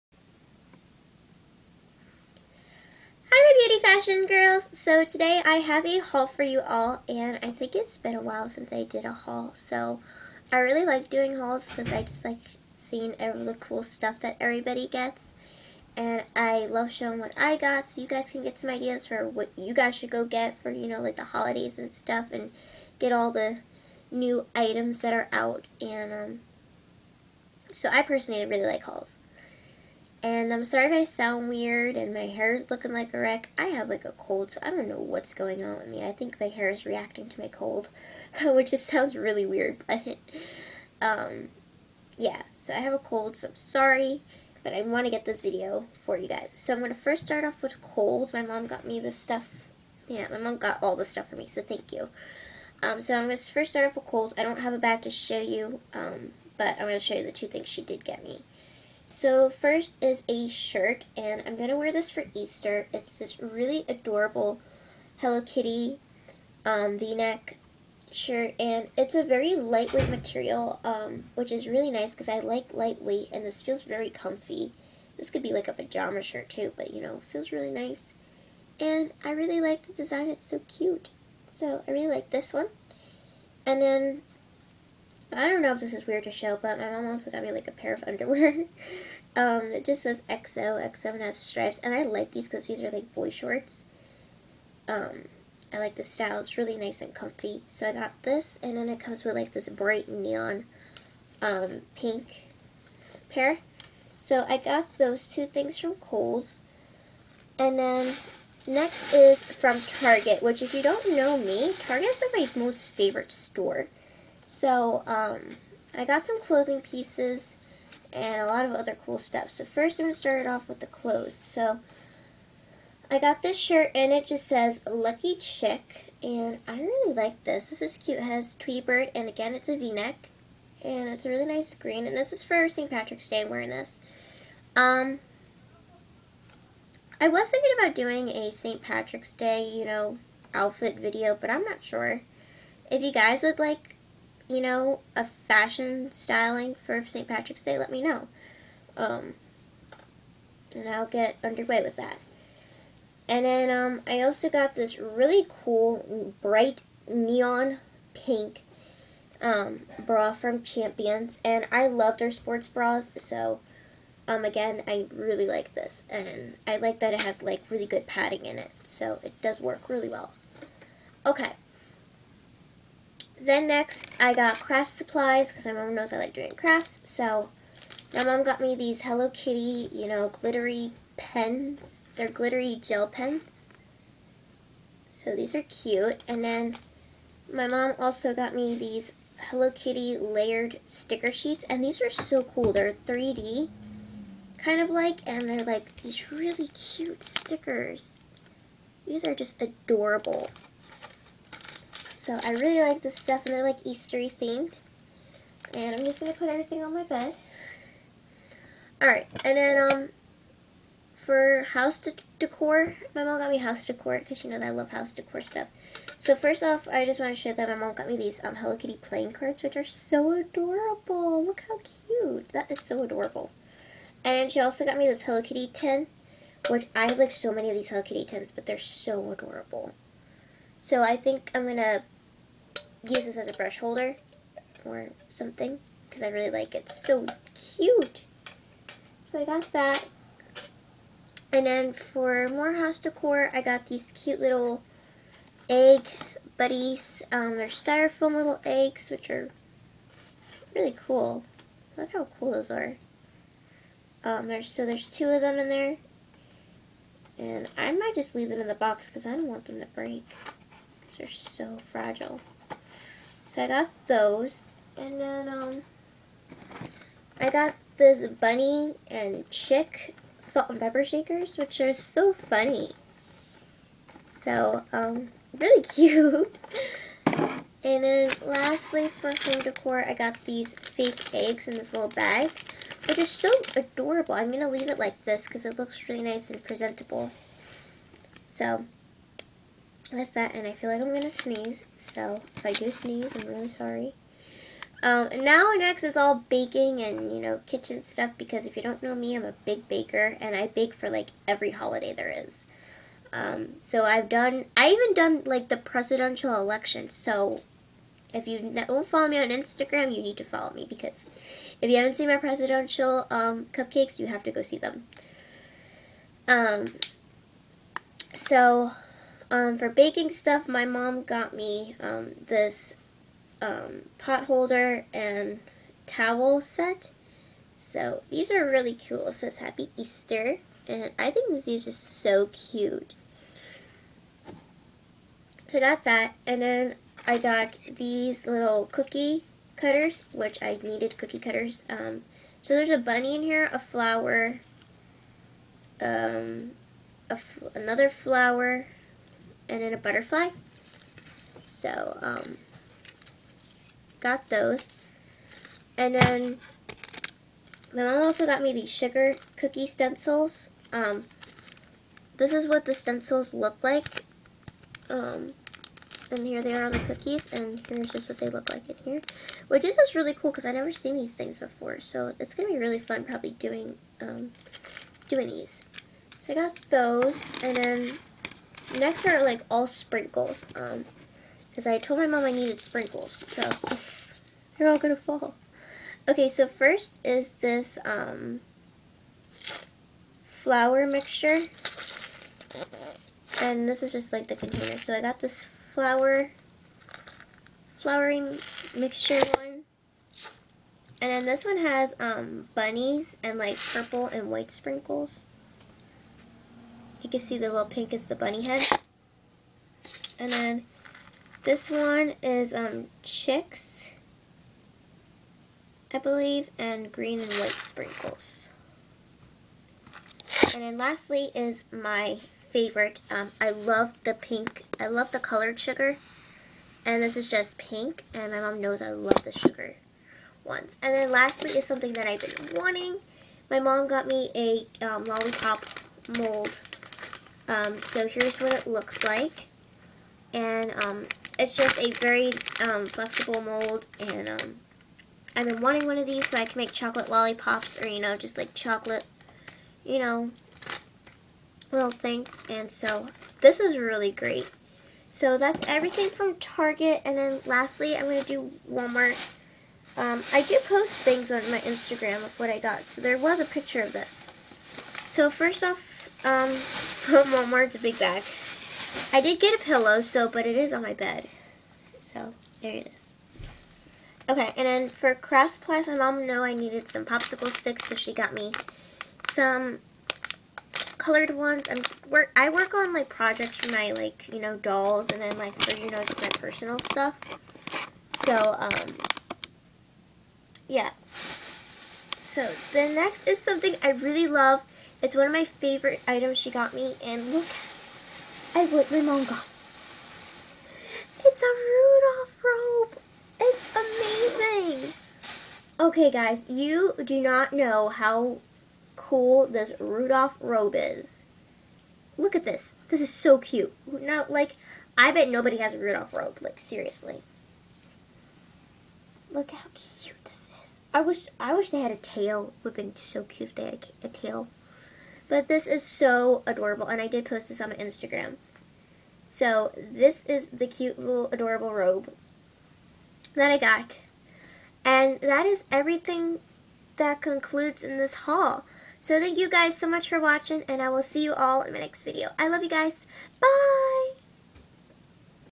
Haul #53: Kohls, Target, & Walmart
Hi, my Beauty Fashion Girls! It's Haul Time. Spring is coming! Yay! My mom got me some cool spring things and I wanted to share what she got me.